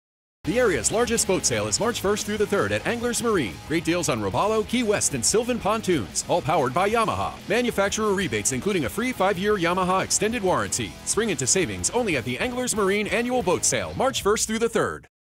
Anglers Marine Annual Boat Sale | March 1st - 3rd 2024
Join us at our Annual Boat Sale, March 1st - 3rd in Supply, NC! Great deals on Robalo, Key West, & Sylvan Pontoons all powered by Yamaha. Manufacturer rebates including a free 5-year Yamaha extended warranty and giveaways all weekend long!